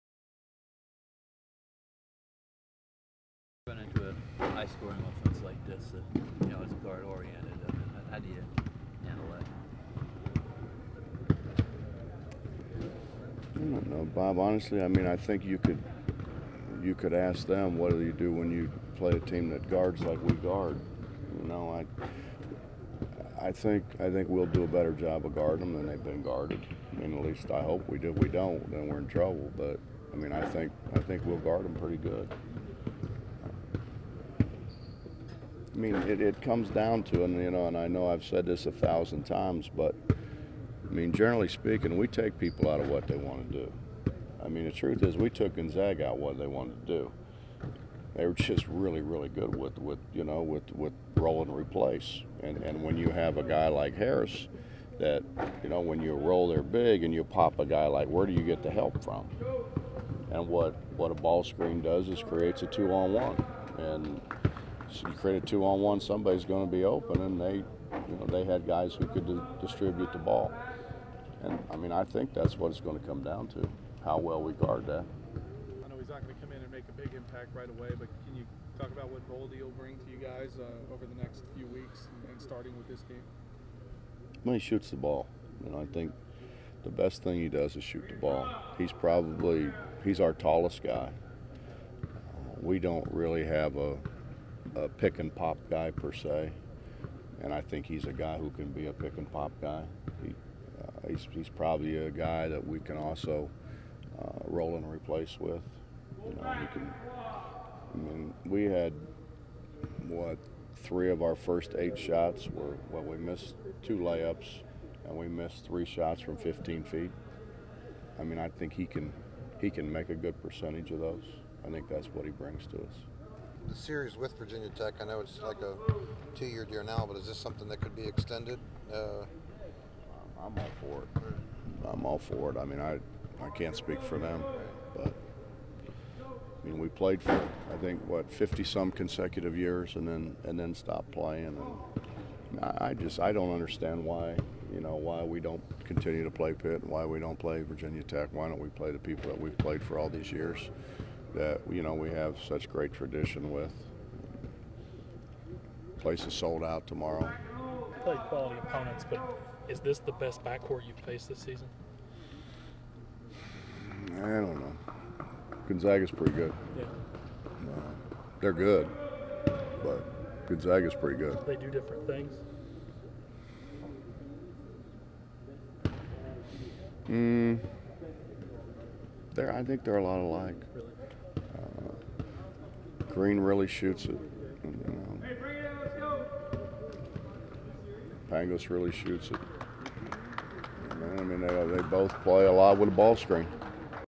Bob Huggins: Virginia Tech preview
Head coach Bob Huggins offers his thoughts on Virginia Tech. The Mountaineers will take on the Hokies on December 8 at 4pm at the WVU Coliseum.